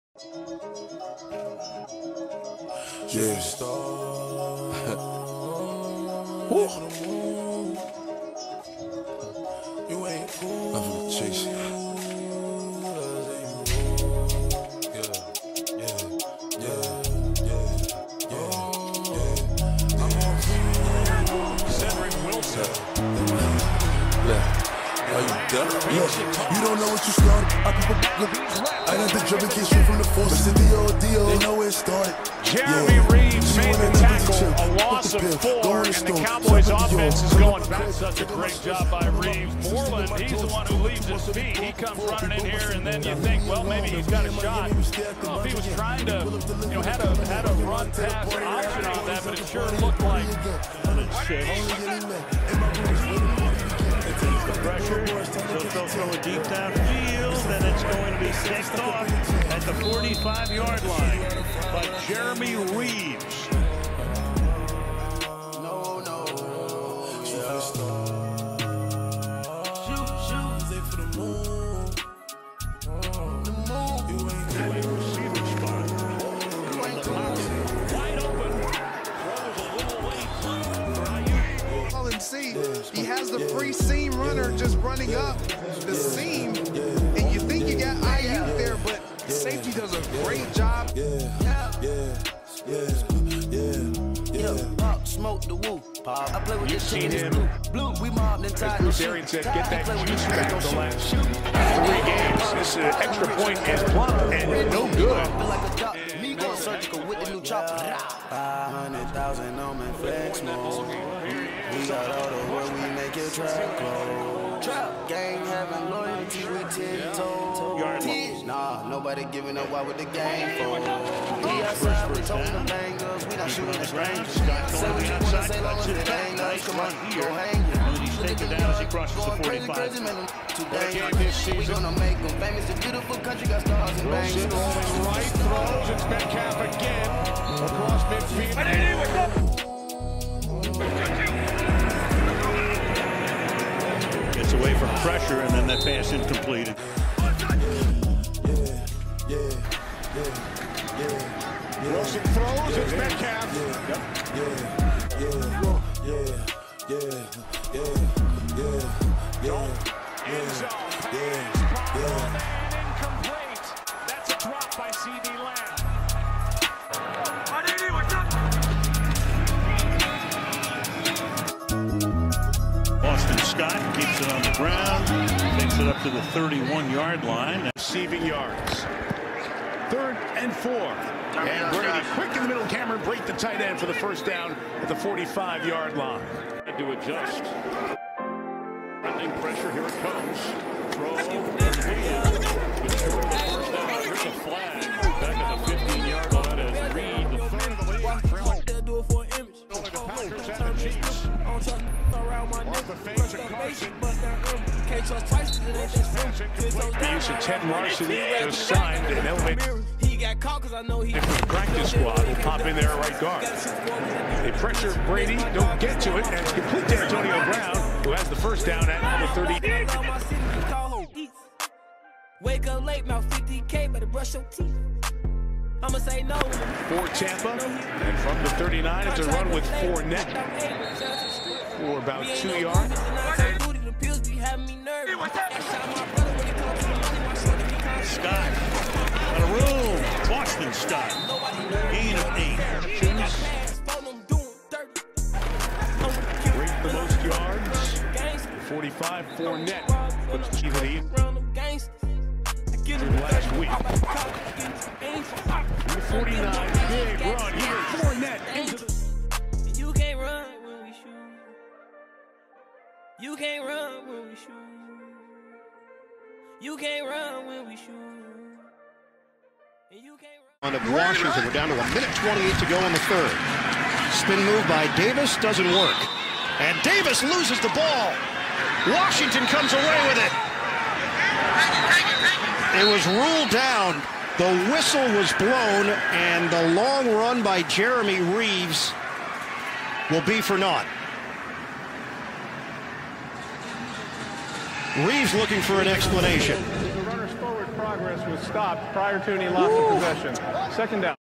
Jeremy Reaves FULL 2020-21 Season Highlights | Breakout Year 💫🎸 | Washington Football Team
Jeremy Reaves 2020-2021 Season Highlights | Breakout Year 👀💫 | Washington Football Team

#39 S 5’11 200

Follow on Instagram: @uttmfb
Follow on Twitter: @UTTMFB